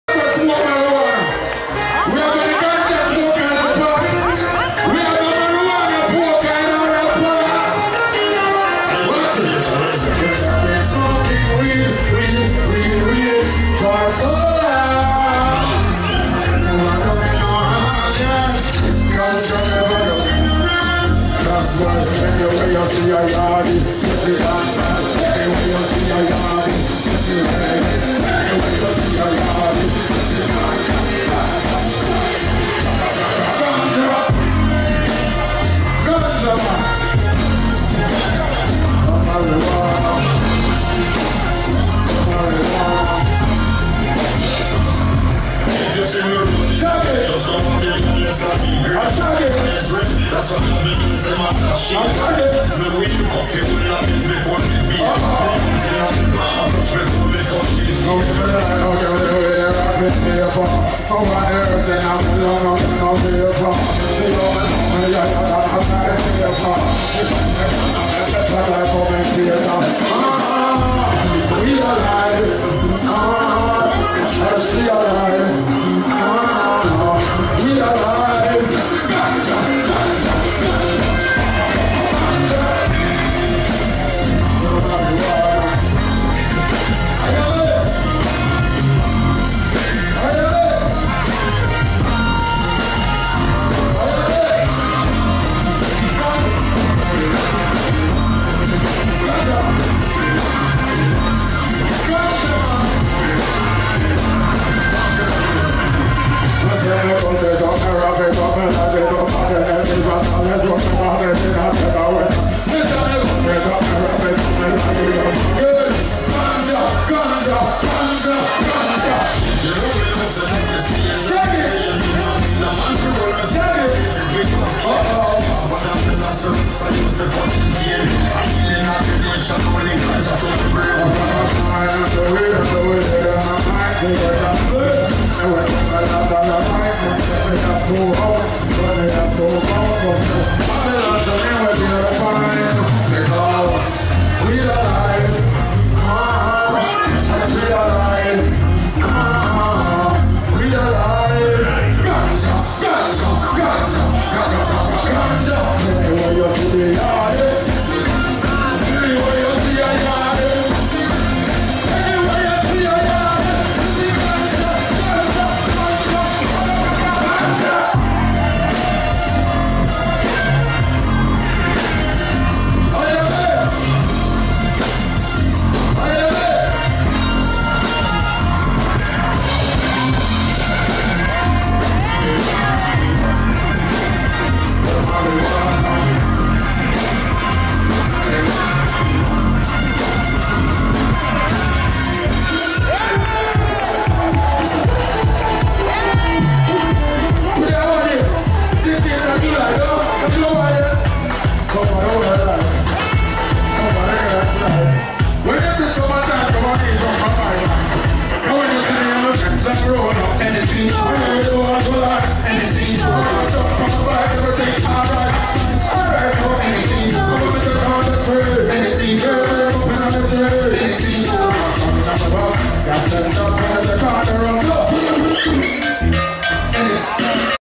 Lambeth Country show @ Brockwell Park 16Jul17 Pt 21 (Bugle)
Lambeth Country Show at Brockwell Park, Herne Hill, London. 16th July, 2017. Backed by the Mafia & Fluxy Band.Part 21. Bugle.